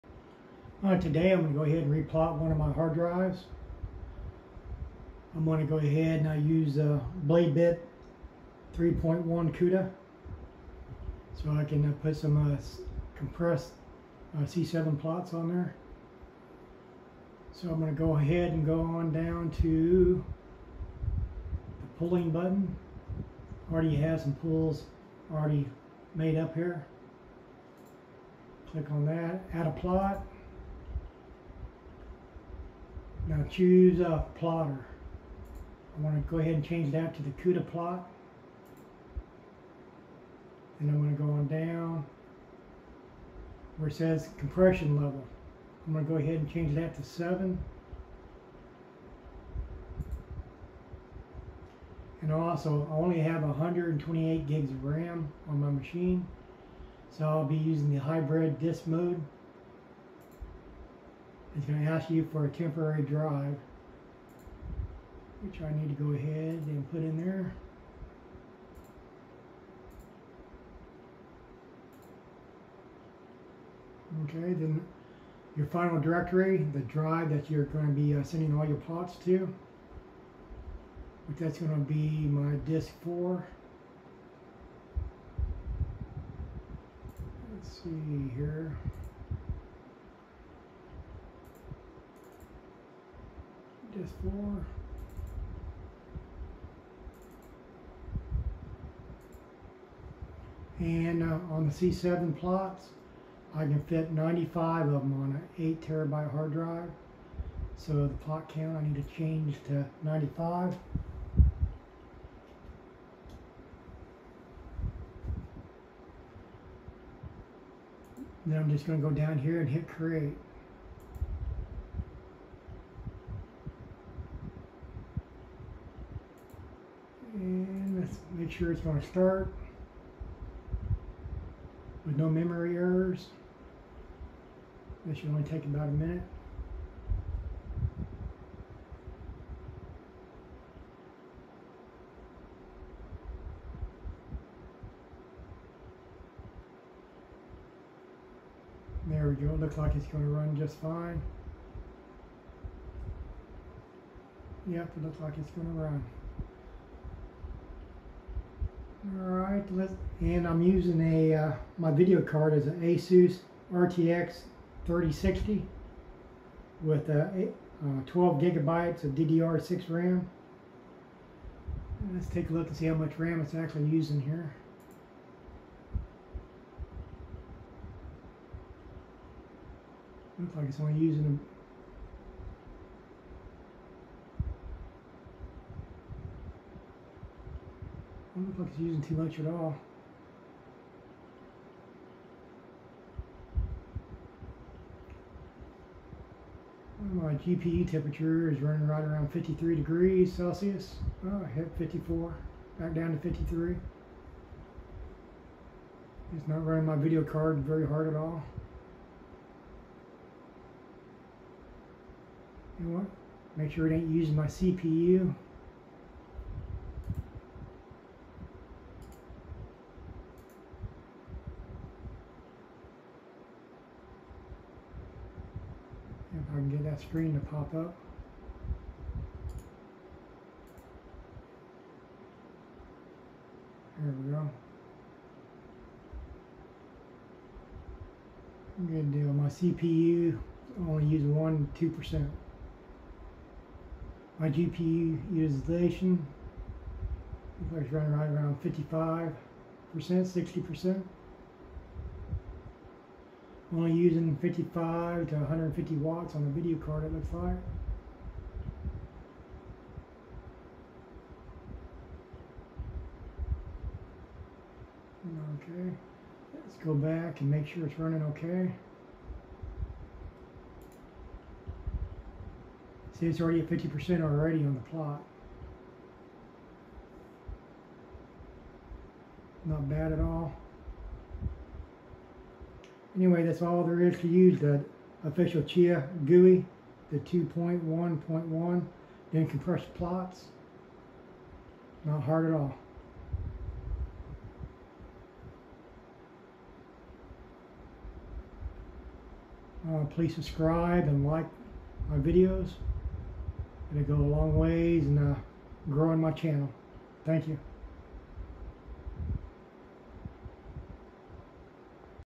Crypto Mining XCH / Chia compressed C7 plotting with 128g DDR5 ram and NVidia RTX 3060
Chia XCH compressed plotting C7 with 128g DDR5 ram and NVidia RTX 3060 video card. On the official Chia GUI 2.1.1